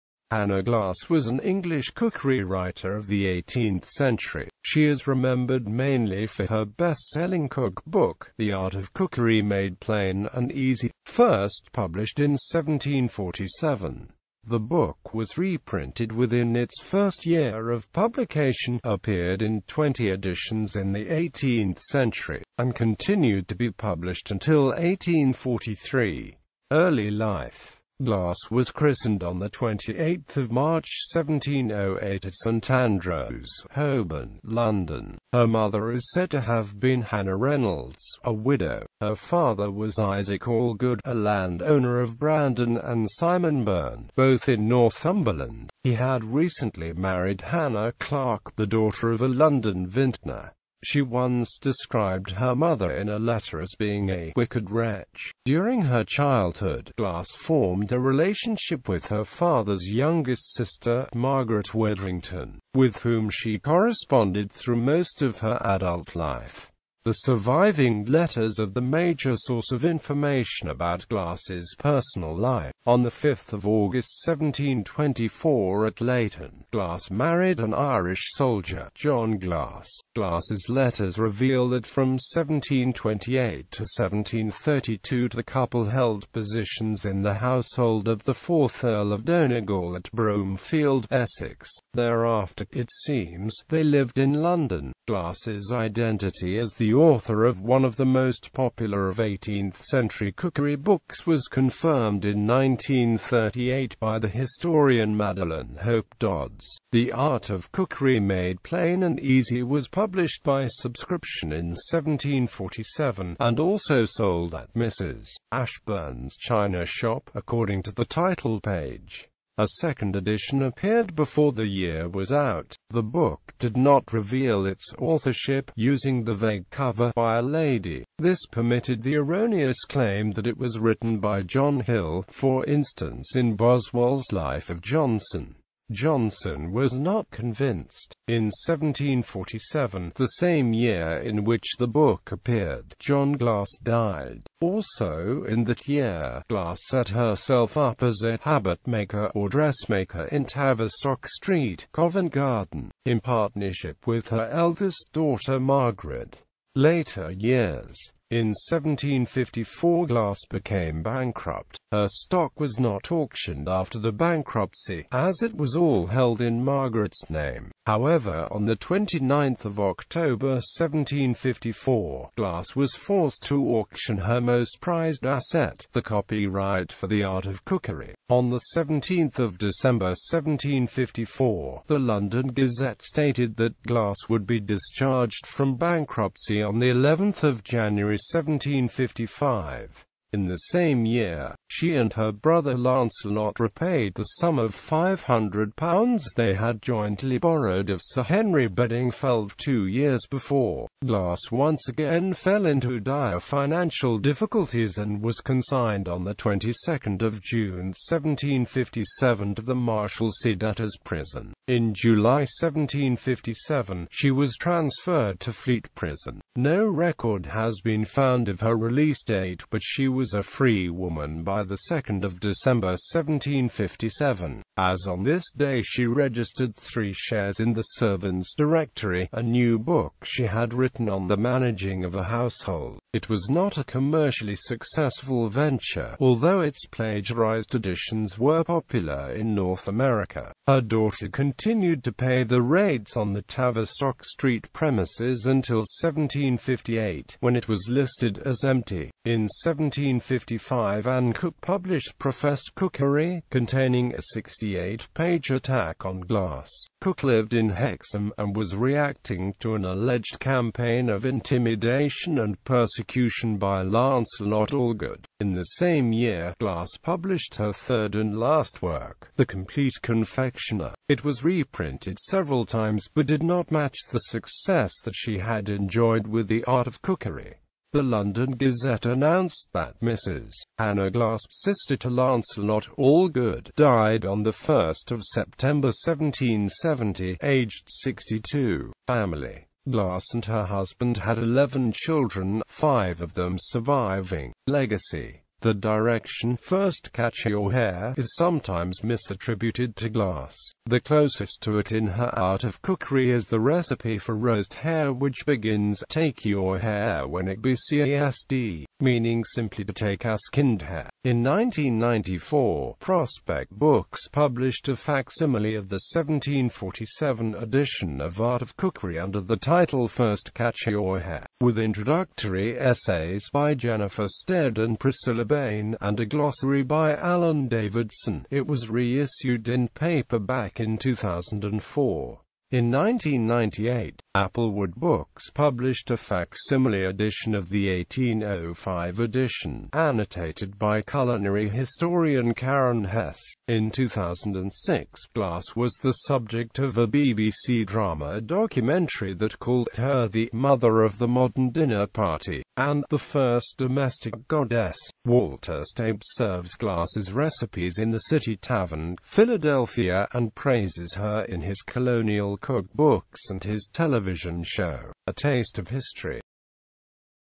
Hannah Glasse
Hannah Glasse was an English cookery writer of the 18th century.She is remembered mainly for her bestselling cookbook, The Art of Cookery made Plain and Easy, first published in 1747.The book was reprinted within its first year of publication, appeared in 20 editions in the 18th century, and continued to be published until 1843.

---Image-Copyright-and-Permission---
About the author(s): Hannah Glasse
---Image-Copyright-and-Permission---